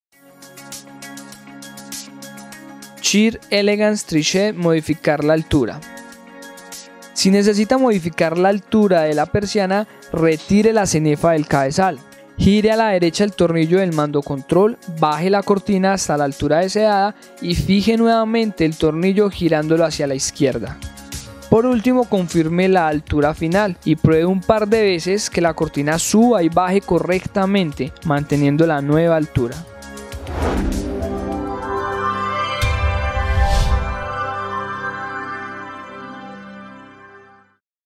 Modificar la Altura de una Sheer Elegance Trishade
En este video se explica el proceso que se debe llevar a cabo para ajustar la altura de una persiana Sheer Elegance Trishade.